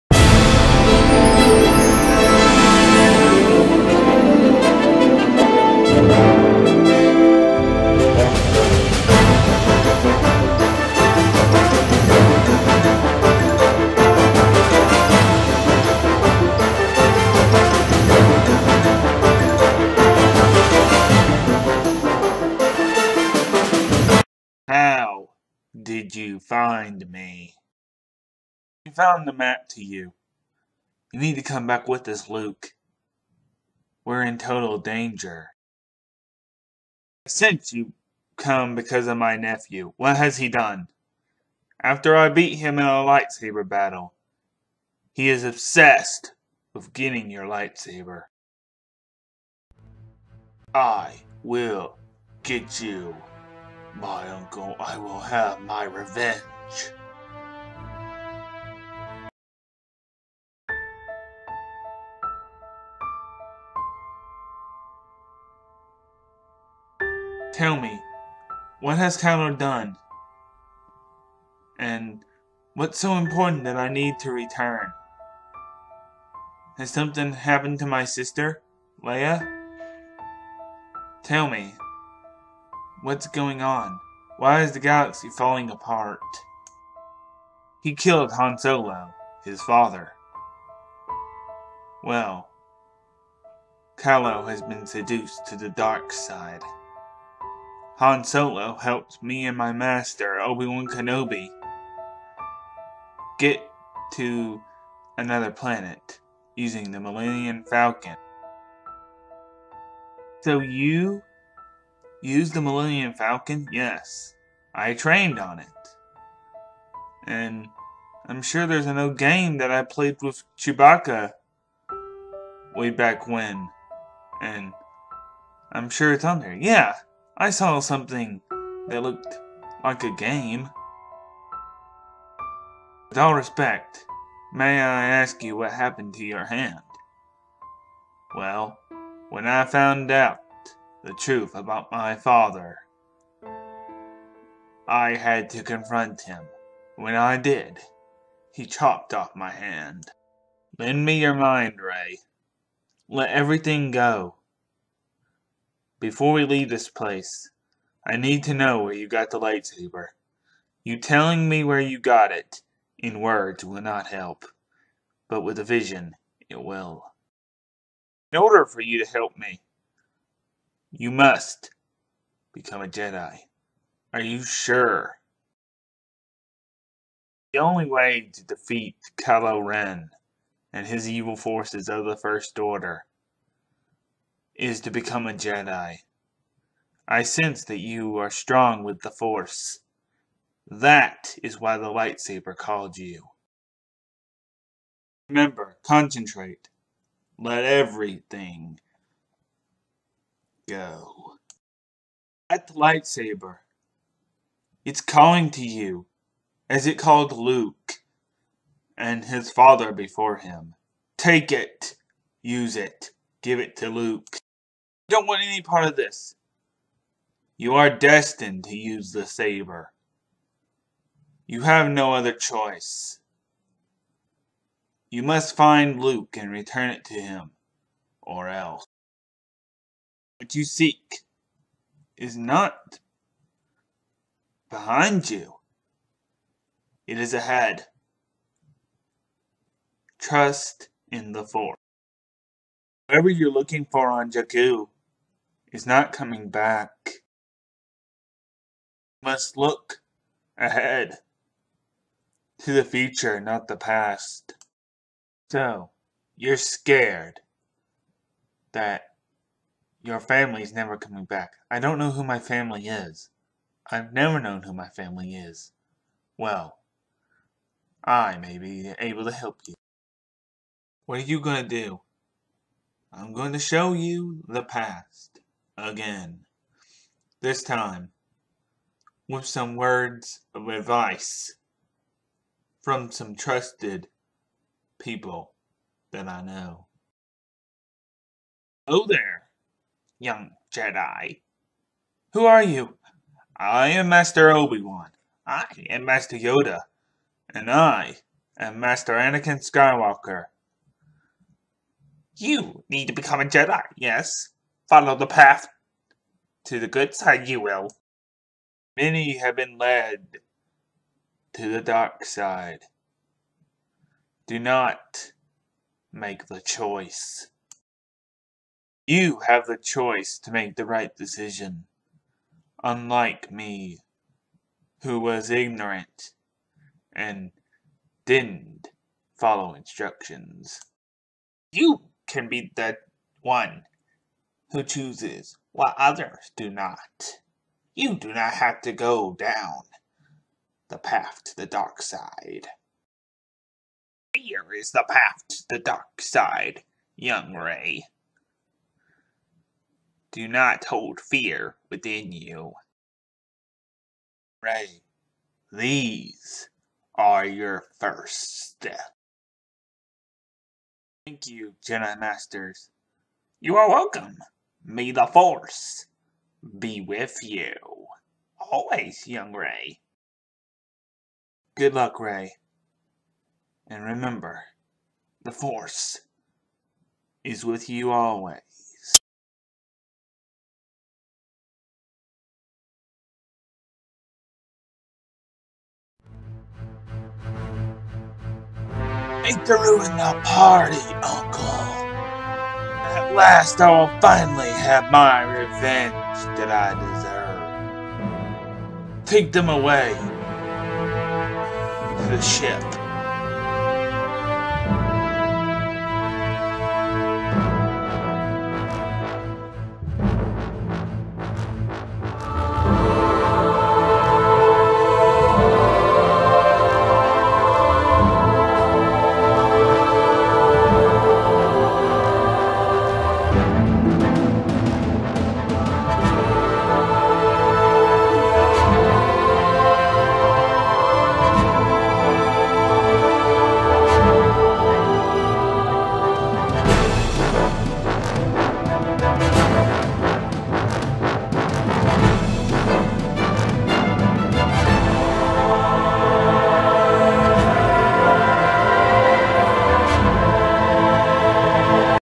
Offical lego star wars episode 8 fan Made Sneak Peak
PLEASE NOTE THERE IS NO WAY THIS IS GOING TO HAPPEN IN 8 NO INFO IN THIS TRAILER IS CONFIRMED EXCEPT FOR A POSSIBILITY FOR REY TO HAVE CONTACT WITH POSSIBLY YODA AND OBI WAN